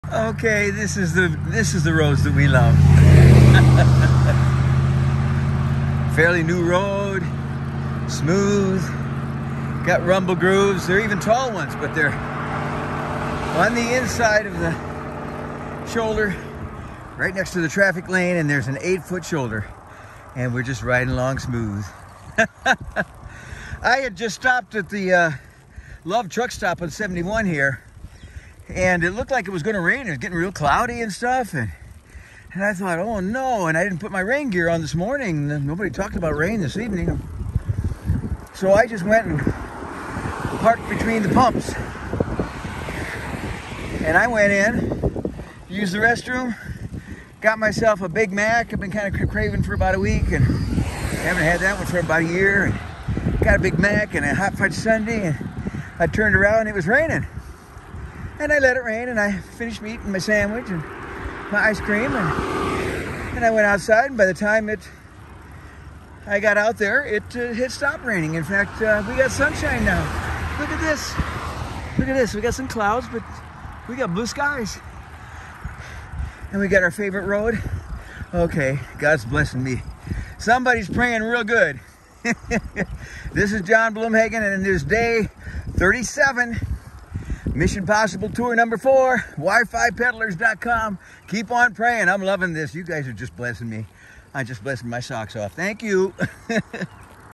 Our favorite style of road
Yes! 4-lane, rumble grooves that are next to the white line, a new smooth surface, and an EiGHT-foot shoulder!

All roads should be like this!